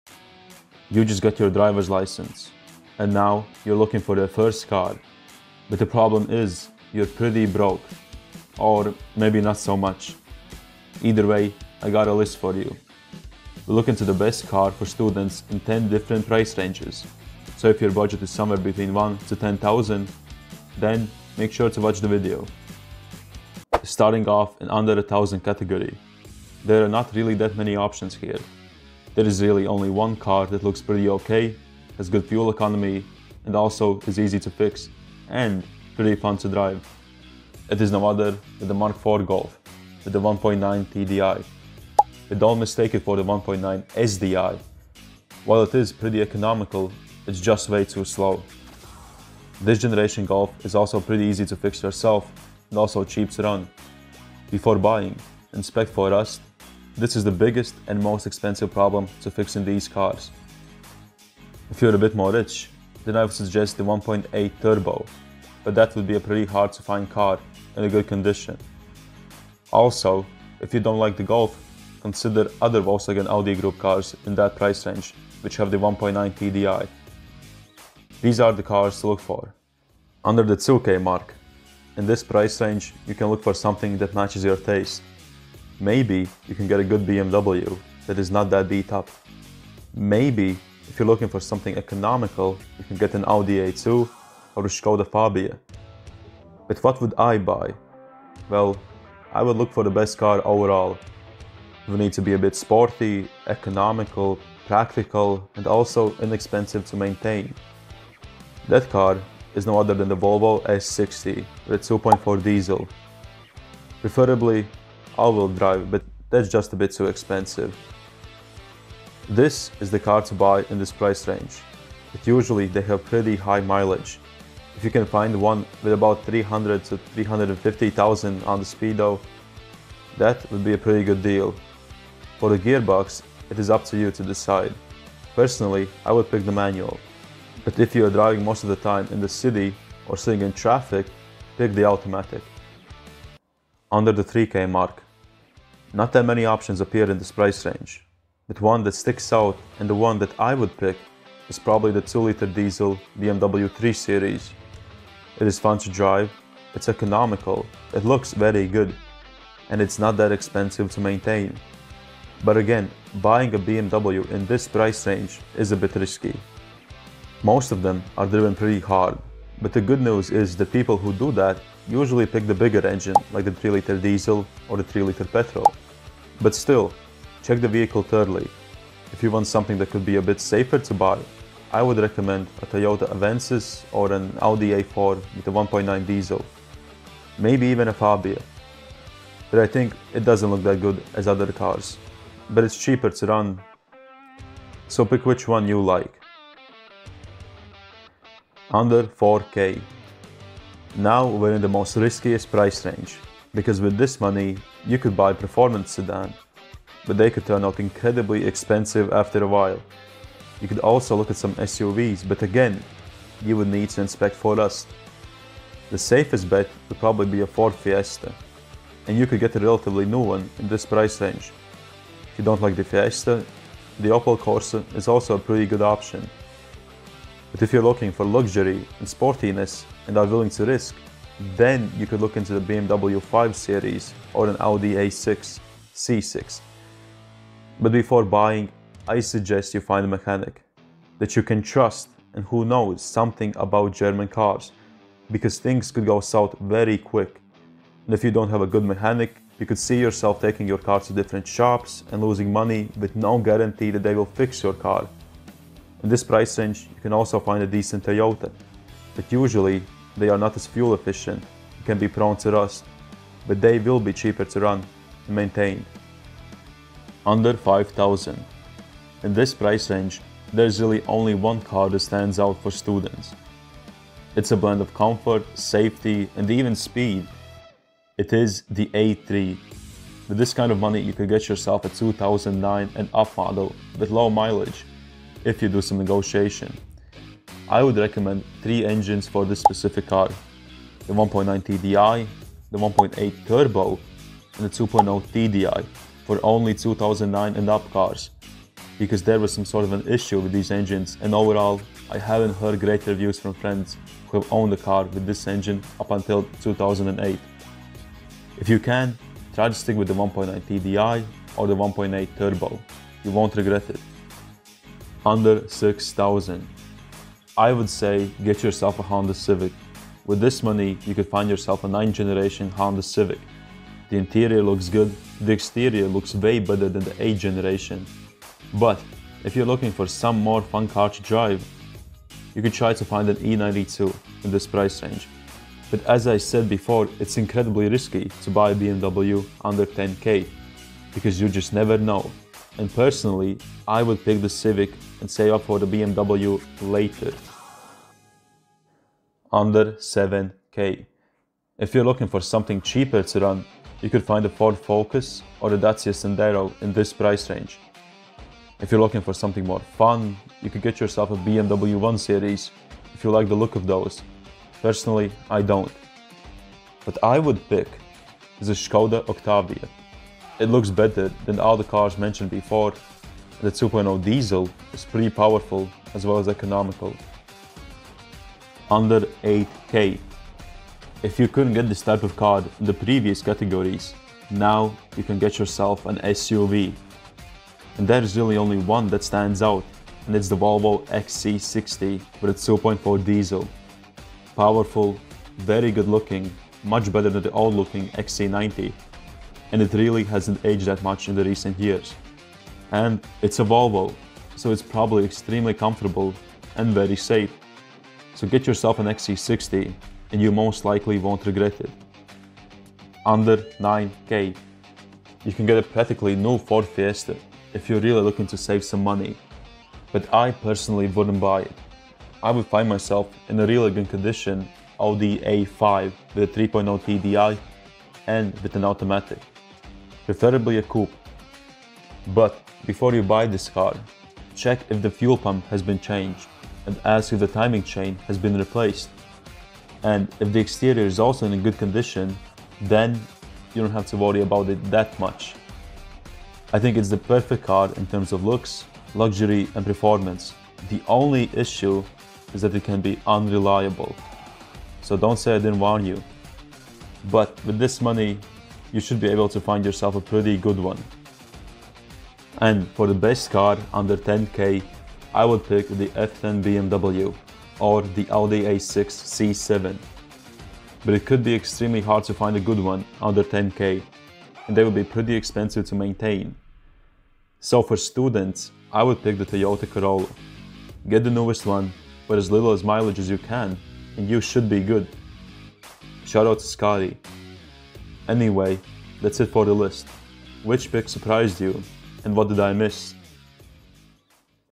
TOP 10 Best First Cars (1k, 2k...10k Price Range)
We’ll look into the best car for students (first car) in 10 different price ranges. So if your budget is somewhere between 1 to 10 thousand - then make sure to watch the video. The list focuses more on european car market, but it some price ranges applies to the US as well.

Time stamps

00:00 Intro
00:23 Under 1k
01:19 Under 2k
02:23 Under 3k
03:17 Under 4k
04:26 Under 5k
05:15 Under 6k
05:49 Under 7k
06:18 Under 8k
06:52 Under 9k
07:41 Under 10k